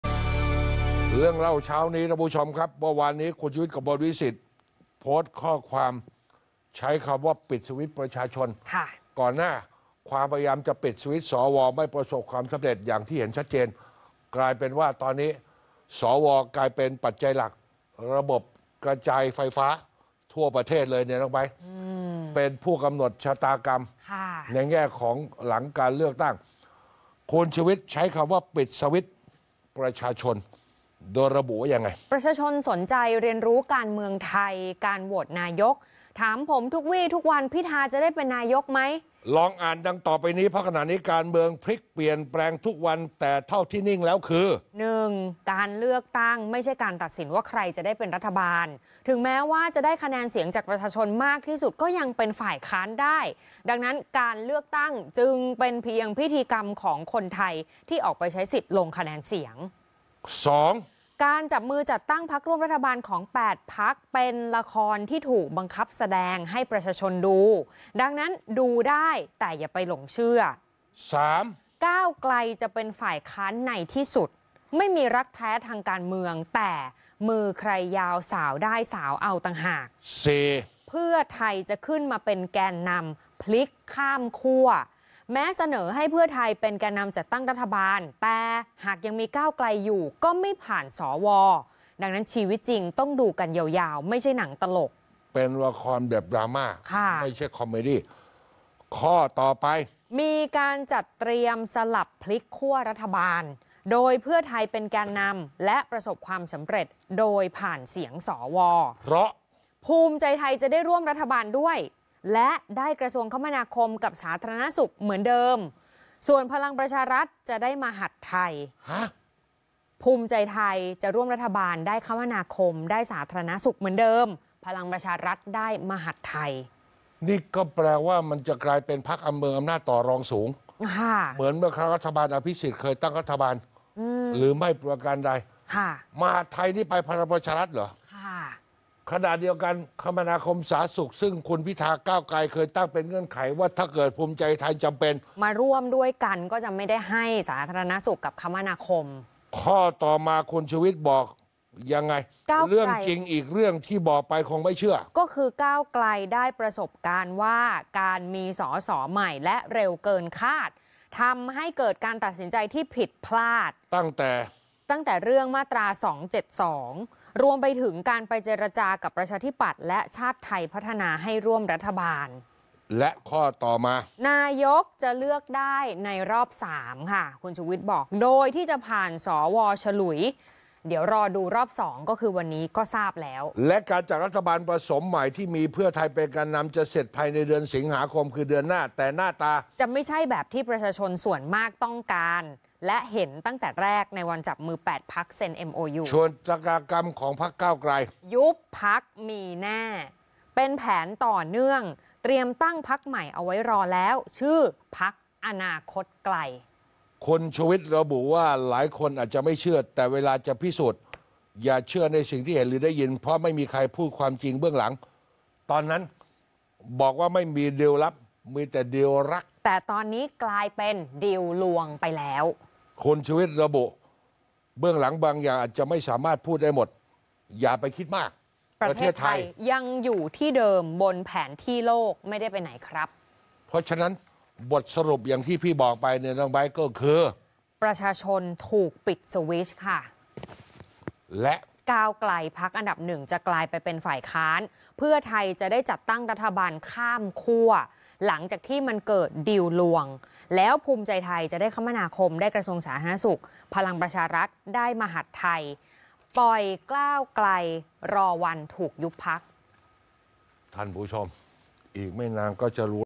'ชูวิทย์' ร่ายยาวทำนายอนาคตการเมืองไทย ฟันธง 'พลิกขั้วรัฐบาล - ก้าวไกลจะเป็นฝ่ายค้าน'
วานนี้ (18 ก.ค. 66) ชูวิทย์ กมลวิศิษฎ์ ได้โพสต์ข้อความ ระบุว่า

[ ปิดสวิตช์ประชาชน ]

ประชาชนสนใจเรียนรู้การเมืองไทย การโหวตนายกฯ ถามผมทุกวี่ทุกวัน “พิธาจะได้เป็นนายกฯ ไหม?” ให้ลองอ่านดังต่อไปนี้ เพราะขณะนี้การเมืองพลิก เปลี่ยนแปลงทุกวัน แต่เท่าที่นิ่งแล้ว คือ

- การเลือกตั้งไม่ใช่การตัดสินว่าใครได้เป็นรัฐบาล แม้ได้คะแนนเสียงจากประชาชนมากสุด ยังเป็นฝ่ายค้านได้ ดังนั้น การเลือกตั้งจึงเป็นเพียงพิธีกรรมของคนไทยที่ออกไปใช้สิทธ์ลงคะแนนเสียง

- การจับมือจัดตั้งพรรคร่วมรัฐบาล 8 พรรค เป็นละครที่ถูกบังคับแสดงให้ประชาชนดู ดังนั้นดูได้ แต่อย่าไปหลงเชื่อ

-ก้าวไกลจะเป็นฝ่ายค้านในที่สุด ไม่มีรักแท้ทางการเมือง แต่มือใครยาวสาวได้สาวเอาต่างหาก

- เพื่อไทยจะขึ้นมาเป็นแกนนำพลิกข้ามขั้ว แม้เสนอให้เพื่อไทยเป็นแกนนำจัดตั้งรัฐบาล แต่หากยังมีก้าวไกลอยู่ ก็ไม่ผ่าน ส.ว ชีวิตจริงต้องดูกันยาวๆ ไม่ใช่หนังตลก

- มีการจัดเตรียมสลับพลิกขั้วรัฐบาลโดยเพื่อไทยเป็นแกนนำ และประสบความสำเร็จ โดยผ่านเสียง ส.ว.

- ภูมิใจไทยจะได้ร่วมรัฐบาล และได้กระทรวงคมนาคม กับสาธารณสุขเหมือนเดิม ส่วนพลังประชารัฐจะได้มหาดไทย เป็นเรื่องจริงอีกเรื่องที่บอกไปก็คงไม่เชื่อ

- ก้าวไกลได้ประสบการณ์ว่า การมี ส.ส. ใหม่ และเร็วเกินคาดทำให้การตัดสินใจผิดพลาด ตั้งแต่เรื่อง ม. 272 หรือการไปเจรจากับประชาธิปัตย์ และชาติไทยพัฒนา ให้ร่วมรัฐบาล

- นายกฯ จะเลือกได้ในรอบ 3 ผ่าน ส.ว. ฉลุย ดูรอบ 2 ก็ทราบแล้ว

- การจัดตั้งรัฐบาลจะเสร็จภายในเดือนสิงหาคม แต่หน้าตาไม่ใช่แบบที่ประชาชนส่วนมากต้องการ และเห็นแต่แรกในวันจับมือ 8 พรรคเซ็น MOU ยุบพรรคมีแน่ เป็นแผนต่อเนื่อง เตรียมตั้งพรรคใหม่ไว้รอ ชื่อพรรคอนาคตไกล

การเมืองหลายเรื่องคนไม่เชื่อ แต่เวลาจะพิสูจน์ว่ามันเป็นเรื่องจริง อย่าเชื่อในสิ่งที่เห็นหรือได้ยิน เพราะไม่มีใครพูดความจริงเบื้องหลัง

ตอนนั้นบอกว่าไม่มี “ดีลลับ” มีแต่ “ดีลรัก” แต่ตอนนี้กลายเป็น “ดีลลวง” ไปแล้ว

ผมพูดไปไม่มีวัตถุประสงค์เชียร์ หรือทำลายใคร แต่ต้องการให้ประชาชนรู้เท่าทันการเมือง แม้ว่าเบื้องหลังบางอย่างผมจะไม่สามารถพูดได้หมด แต่หากเดาจากสิ่งที่ผมพูดไปก่อนหน้านี้ประชาชนที่ติดตามคงทราบ

อย่าไปคิดมาก ประเทศไทยยังอยู่ที่เดิมบนแผนที่โลกไม่ได้ไปไหนครับ